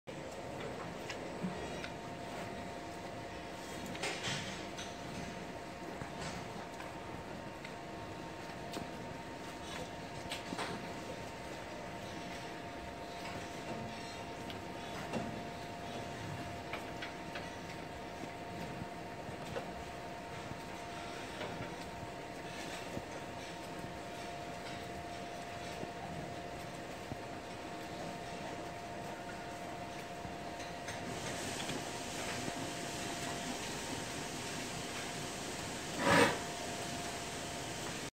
Cow Cleaning its nose after eating in the AMS Galaxy milking robots
If you want to see the AMS Galaxy milking robots in action go to my YouTube channel or AMS Galaxy's YouTube channel or visit their web